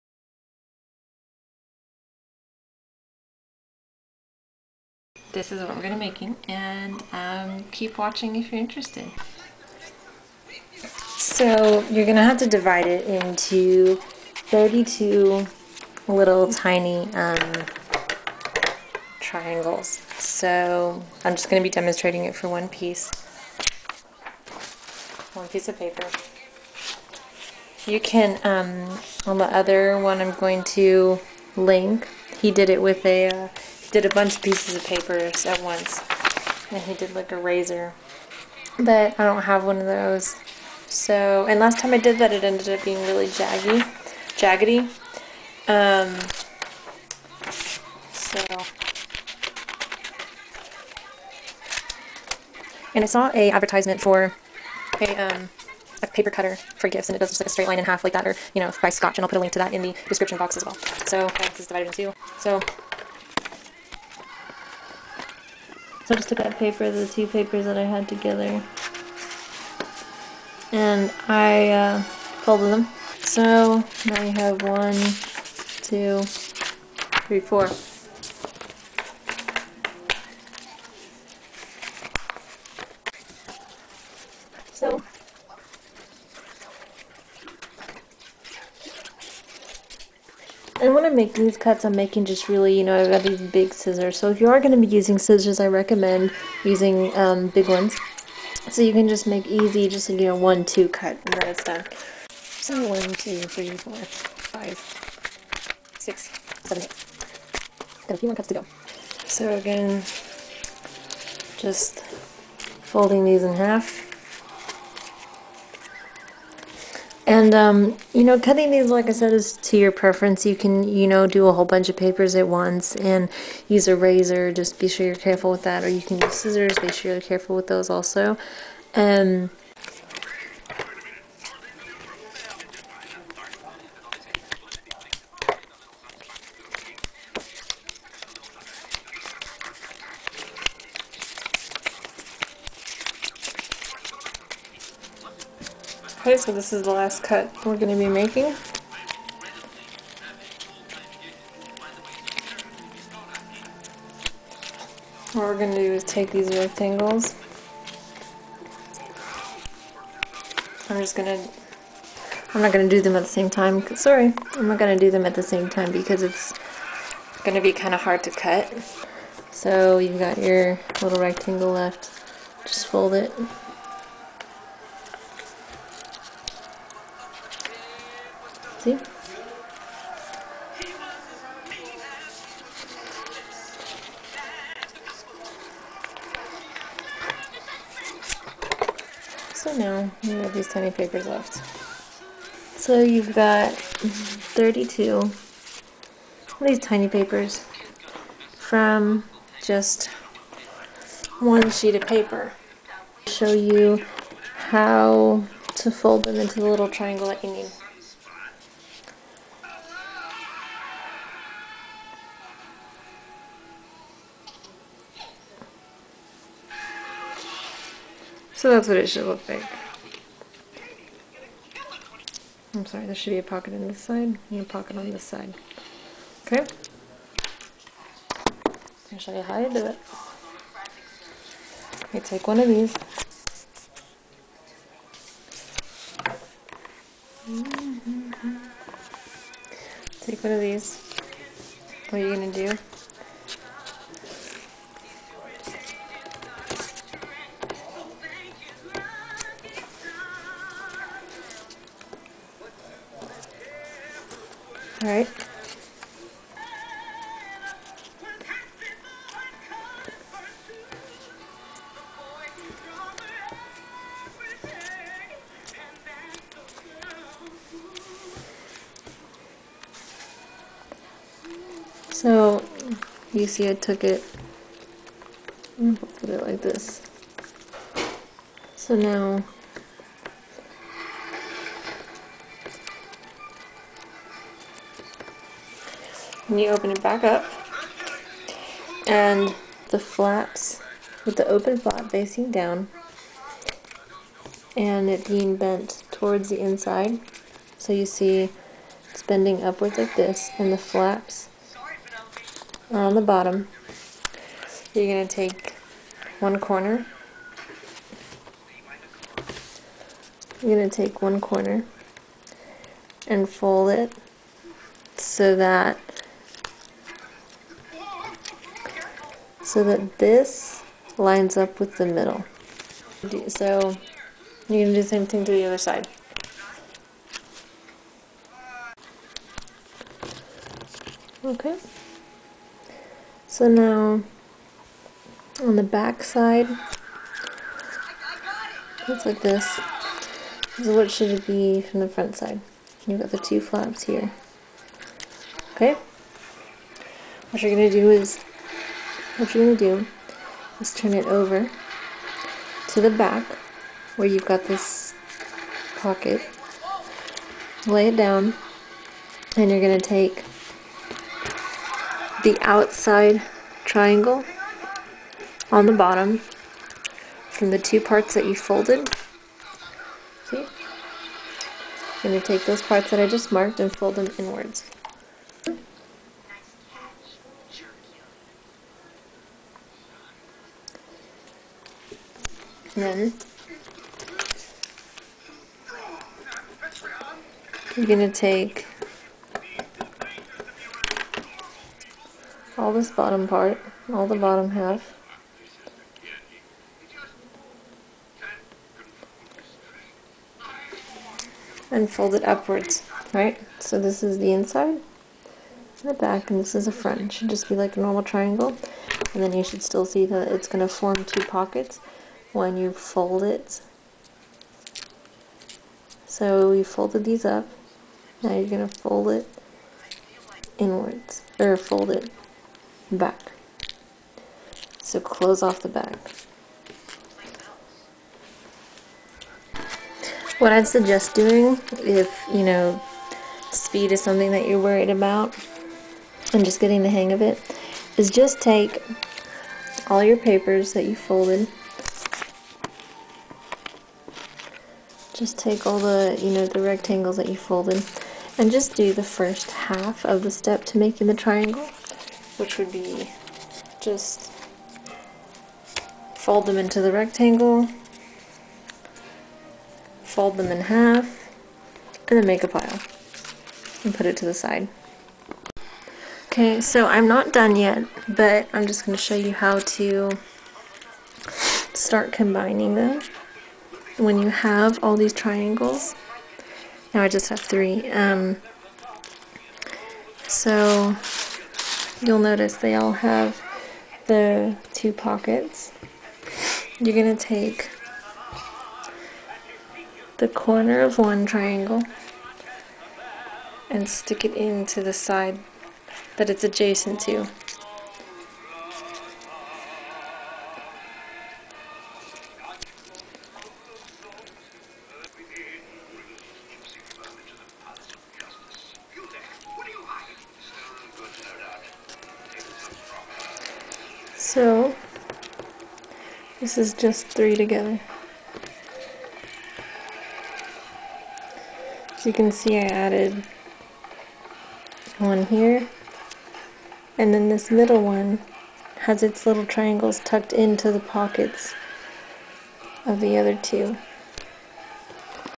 Origami Swan Part I
In this first part you will see:
1. How to fold/cut a sheet of paper to get 32 slips
2. How to fold the slips to make triangles
3. How to attach the triangles

Which has:
1. Explanation of the layers
2. MORE explanation of how to connect the triangles
3. The Wings
4. The Tail

Which has:
1. The Neck
2. Layers Explained Again
3. How I did the colored parts
***You will need:
2 piles of 30 (body base)
4 piles of 15 (body)
2 piles of 15 (wings)
1 pile of 6 (tail)
1 pile of 15ish (neck length to your preference)